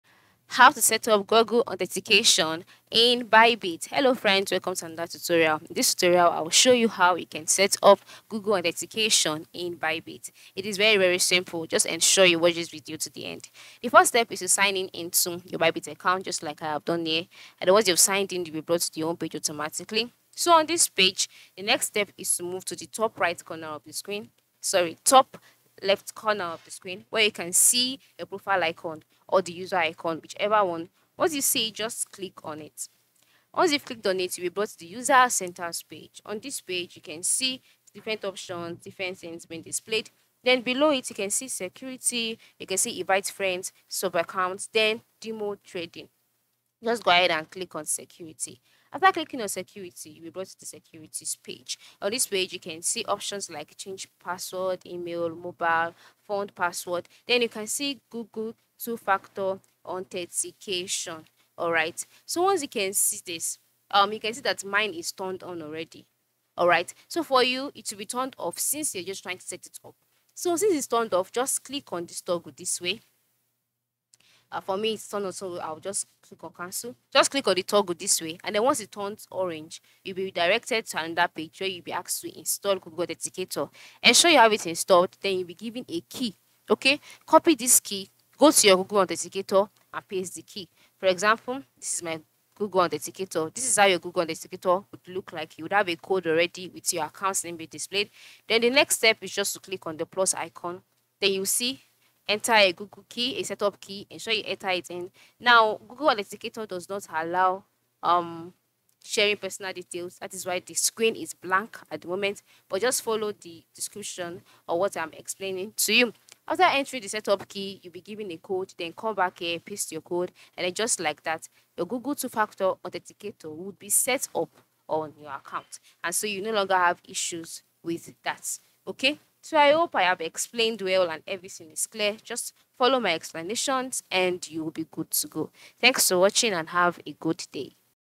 How To Setup Google Authenticator In Bybit (Quick Tutorial)
Today we talk about How To Setup Google Authenticator In Bybit, so stay until the end of the video to see the full explanation.